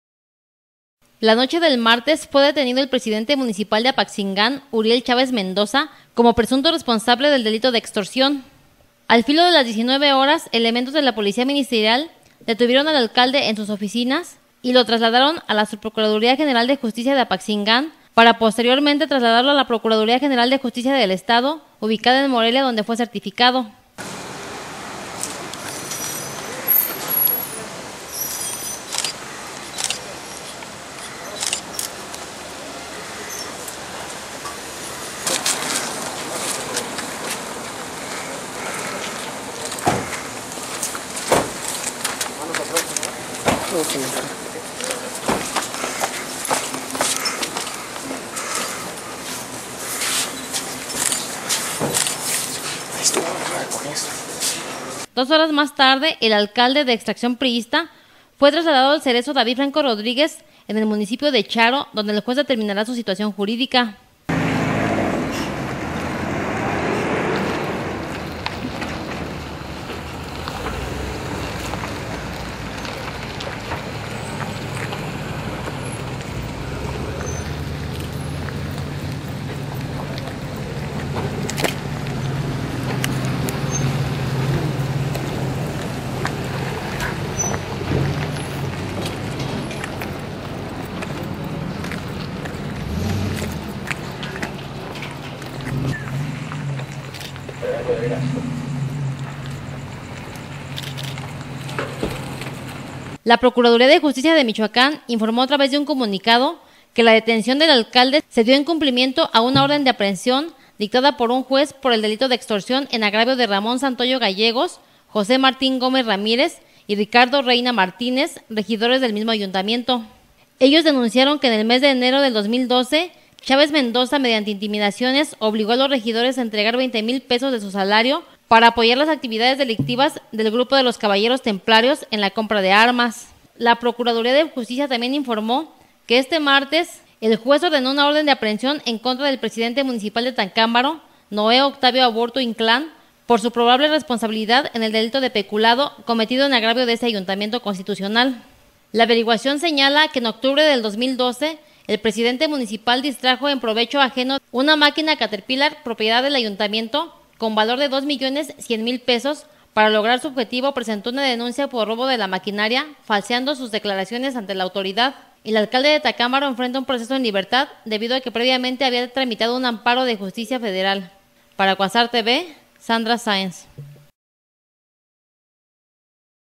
Detienen a edil de Apatzingán, el de Tacámbaro amparado
La noche del martes fue detenido el presidente municipal de Apatzingán, Uriel Chávez Mendoza, como presunto responsable del delito de extorsión en contra de tres regidores a quienes obligó entregar dinero para apoyo de un grupo delictivo...